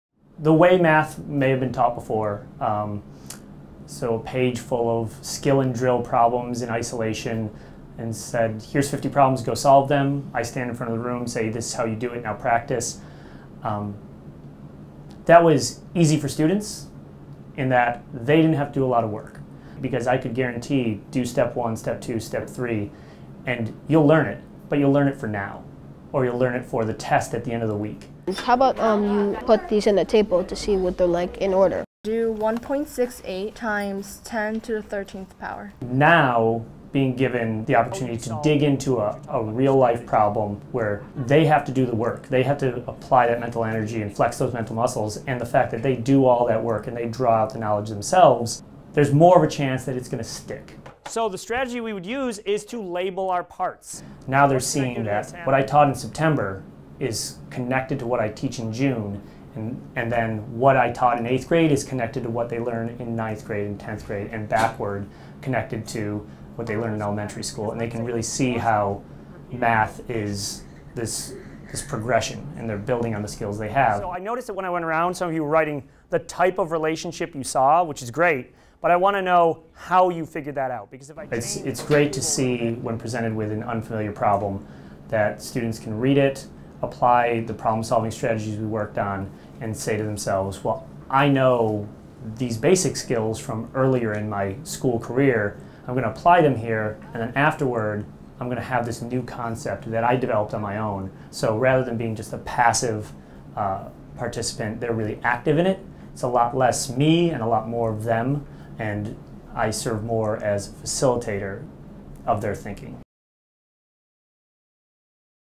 Video Blog: Math Before and After Common Core
"Skill and drill." That's the way it used to be, says math teacher Peter Schmitt of the Lower Manhattan Community Middle School. Listen to how he describes the change in his classroom with Common Core Learning Standards, and how it's affecting students and his own role as educator.